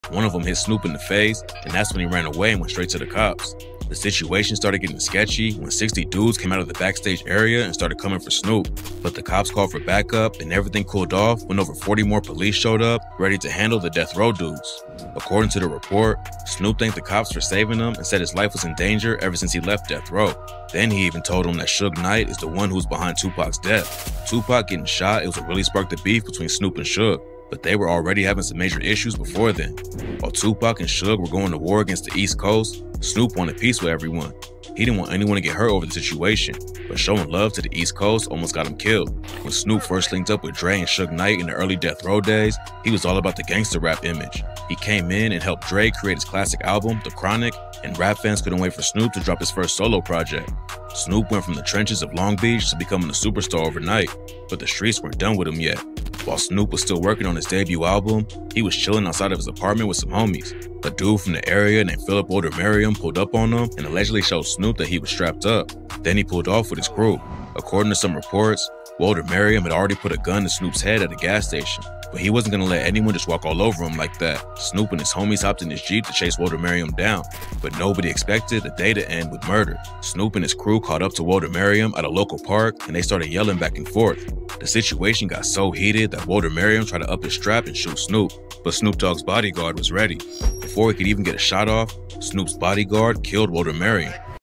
Snoop Dogg's Shocking Claim: Suge Knight's Role in Tupac's Death
🔥 Snoop Dogg's revelation shakes the foundations! Delve into the intense drama as Snoop points fingers at Suge Knight, hinting at his involvement in Tupac's tragic death. Explore backstage clashes, post-Death Row dangers, and the untold story behind 2Pac's shooting. 🎤🕵️‍♂️